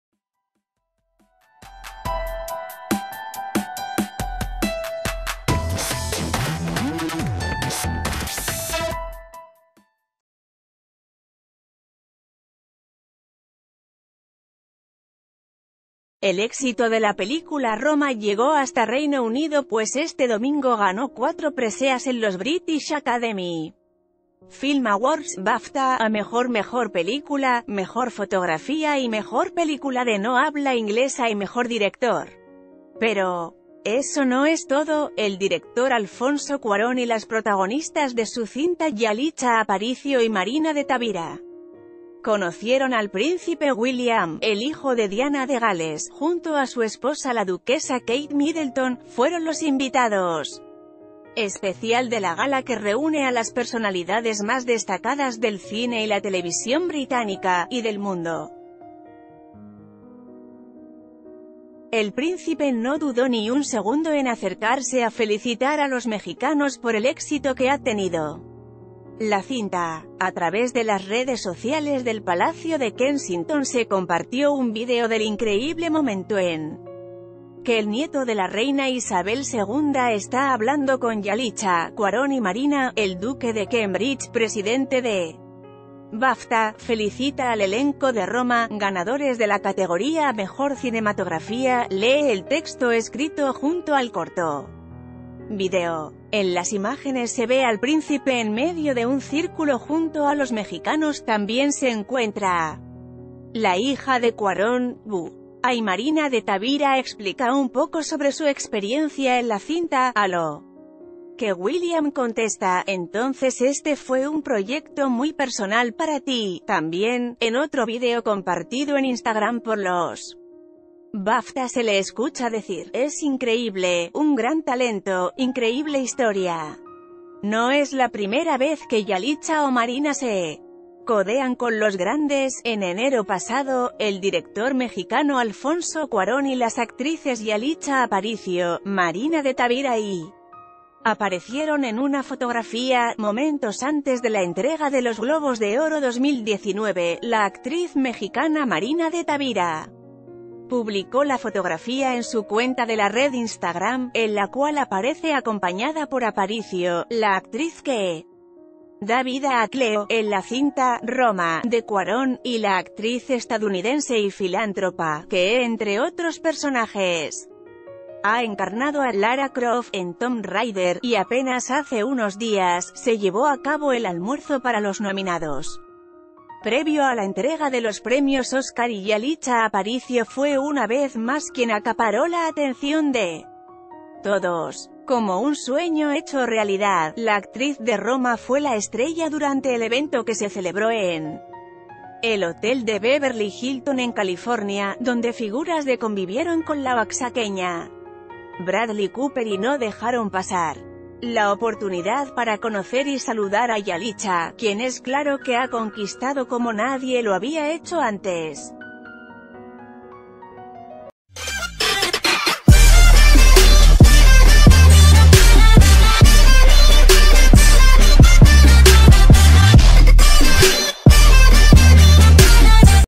Así fue el increíble encuentro de Yalitza Aparicio con el príncipe William
El éxito de la película Roma llegó hasta Reino Unido pues este domingo ganó cuatro preseas en los British Academy Film Awards (BAFTA) a Mejor Mejor Película, Mejor Fotografía y Mejor Película de no habla inglesa y Mejor Director. Pero eso no es todo, el director Alfonso Cuarón y las protagonistas de...